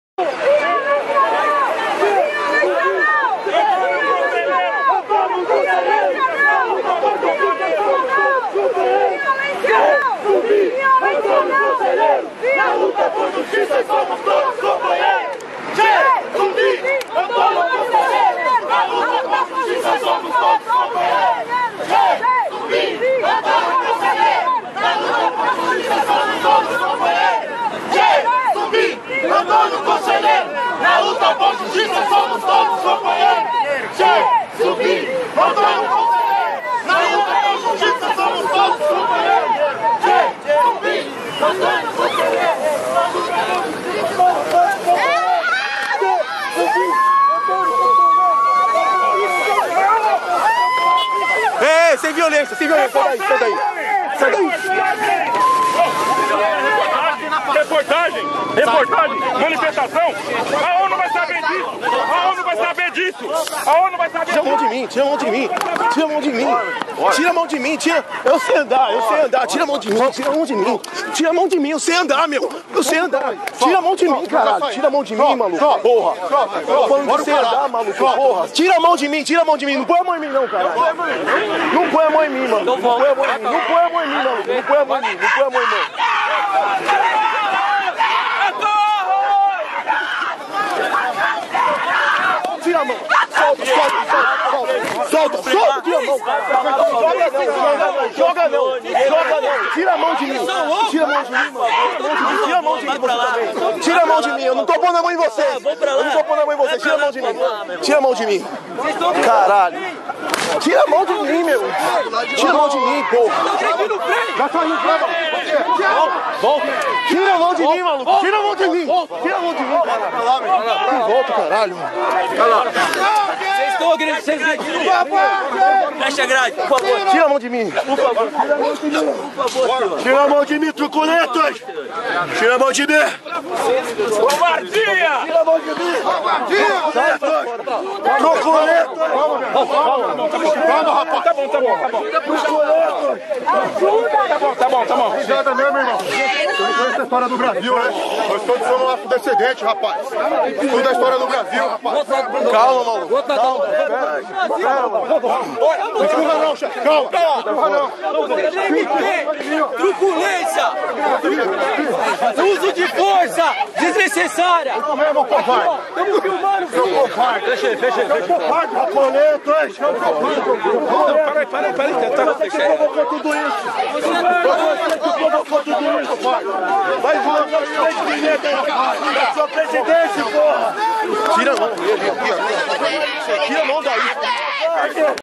Agentes espancam covardemente Frei Franciscano, mulheres e homens militantes da Educafro
Na data de 26 de junho de 2013, em Frente ao Palácio do Planalto , cerca de 100 agentes que fazem a guarda do palácio e da Presidente da Republica espancaram covardemente um grupo de 20 pessoas composto por Frei Franciscano, mulheres e homens militantes da Educafro que realizavam protesto pacífico no local. ATENÇÃO!! As imagens são fortes e irão chocar muitas pessoas, mas é a nossa atual triste realidade onde a ditadura do "CALE-SE" insiste em mostrar suas garras.